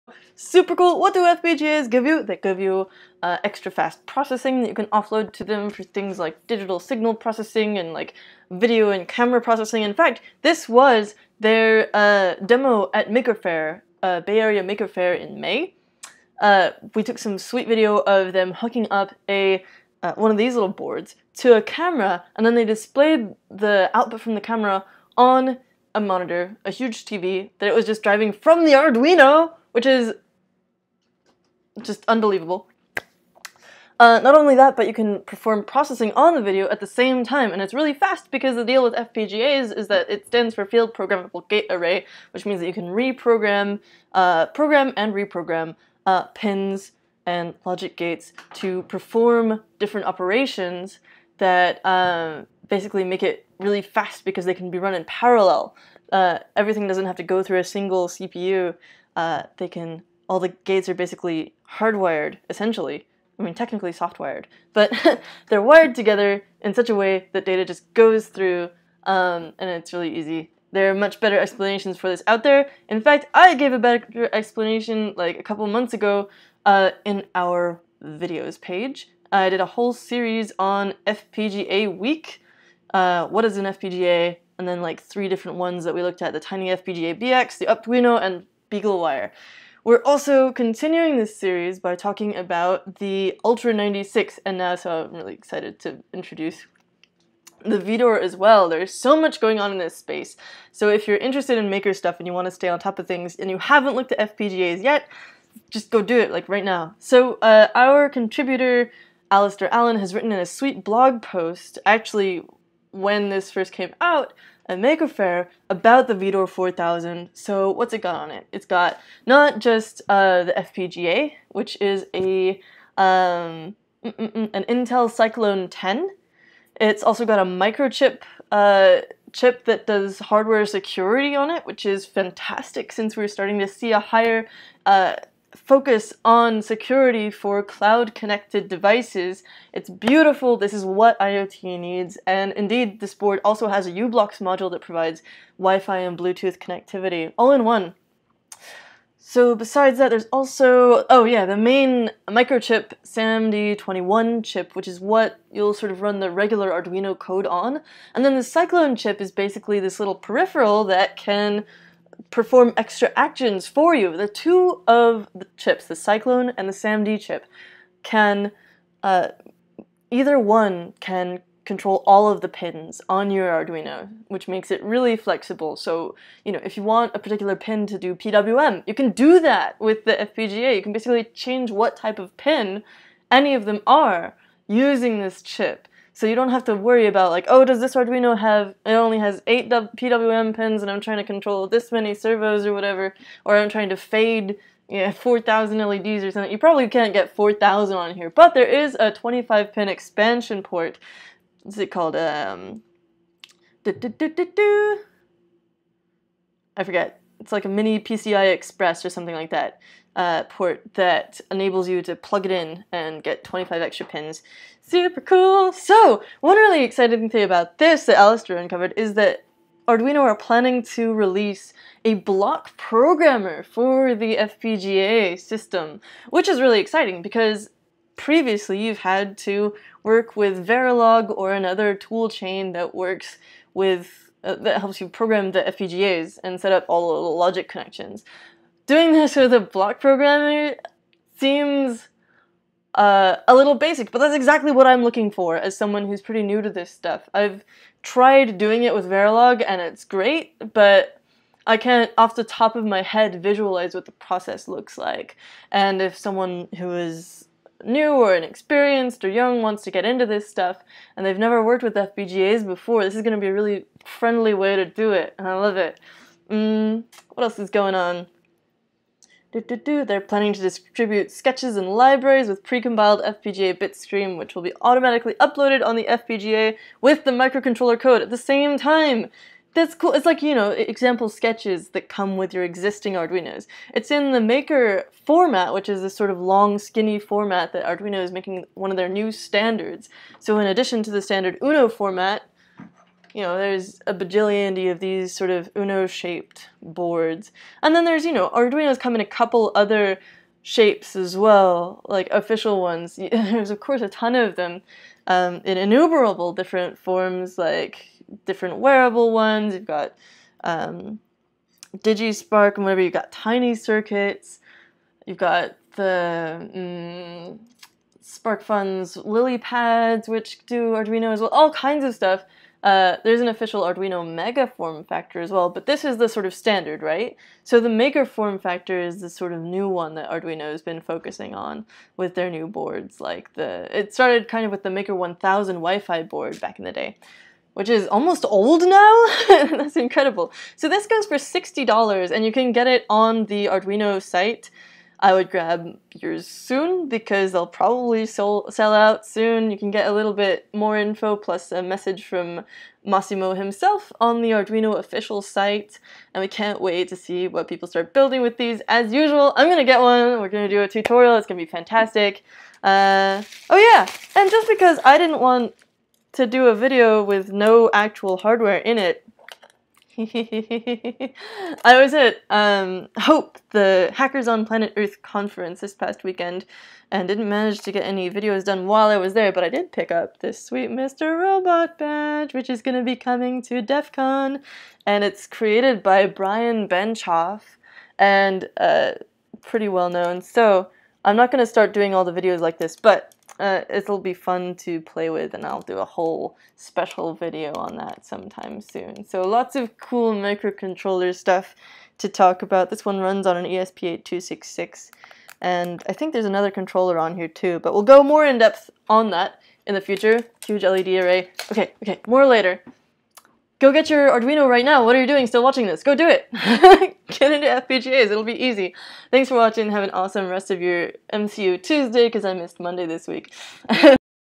Available Now! Arduino MKR Vidor 4000
The first Arduino FPGA board is out! Get the MKR Vidor 4000 now for $60